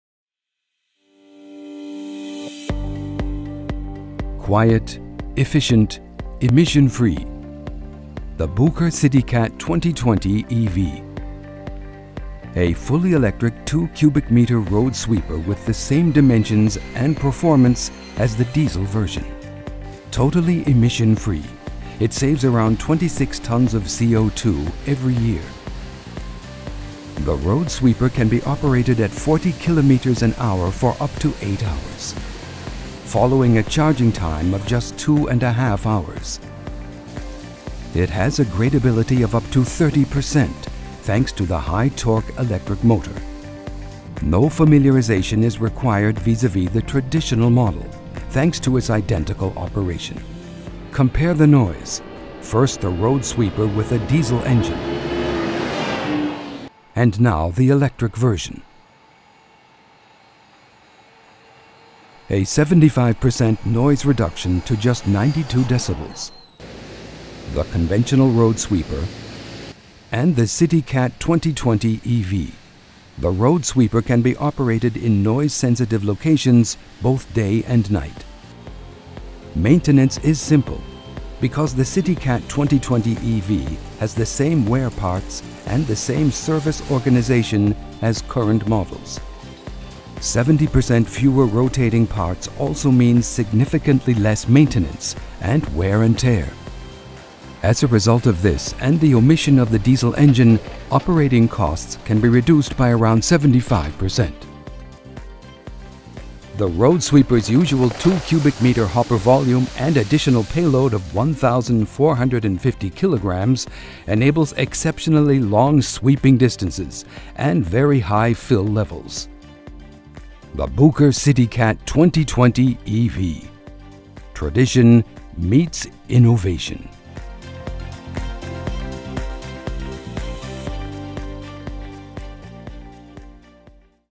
CityCat 2020ev Electric Compact Sweeper
The Bucher CityCat 2020ev is the first all-electric compact sweeper in the 2m³ range. The machine sets new standards in terms of noise reduction.
Zero-emission and efficiency without compromising the performance.

Zero Emission The CityCat 2020ev makes sustainable street cleaning a reality: ultra-quiet, emission-free, people- and environmentally-friendly road sweeping. Operating noise under 92 dB(A) Due to the very low noise emissions, operations are possible around the clock, even at night.

The CityCat 2020ev is around 10 dB(A) quieter than conventional sweepers. Lower operating costs A special objective was to unify ecology and economy. Therefore, the CityCat 2020ev is significantly quieter, has zero emissions and is more cost efficient as sweepers with a diesel engine.

Thanks to the drop out of fossil fuels, lower maintenance, service and repair costs, the operating costs are around 75 % lower than with conventional diesel vehicles. Increase in efficiency the exceptionally robust and maintenance-free design provides a long service life and due to the liquid cooling and heating a minimized battery aging. Familiar Service the CityCat 2020ev requires the same service- and wearing parts as the conventional CityCat 2020.

The parts can be ordered as usual, through the Bucher Municipal eShop . Our service department will guarantee a fast service , downtime will be reduced to a minimum.